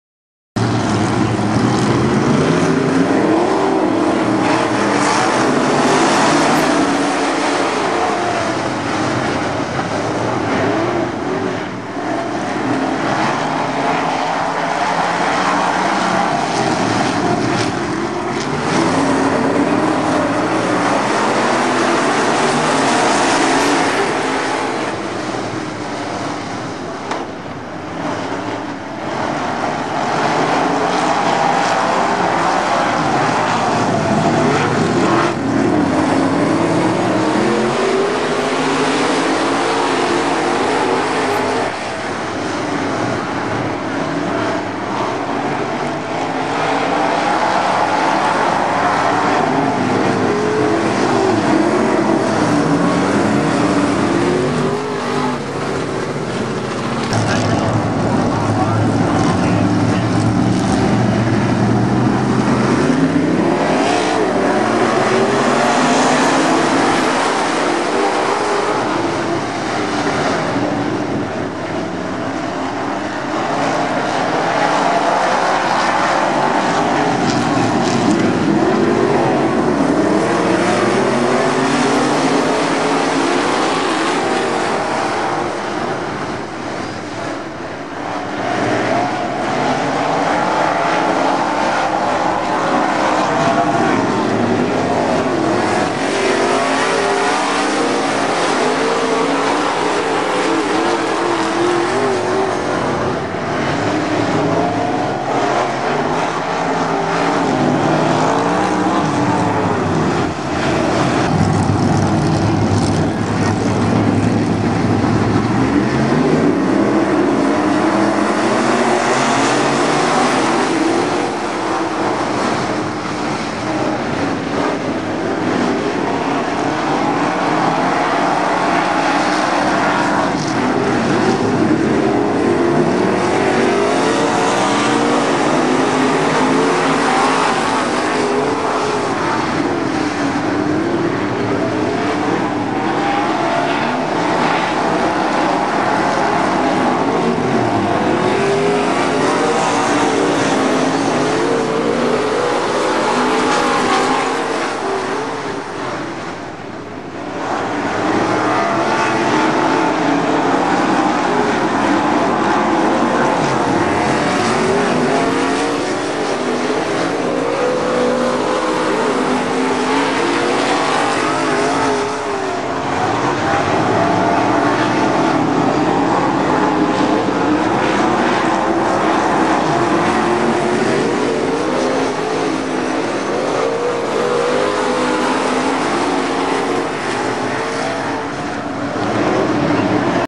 Modifieds - Heat #1
Heat #1 of the USRA Modifieds at Cowtown Speedway on April 28, 2012.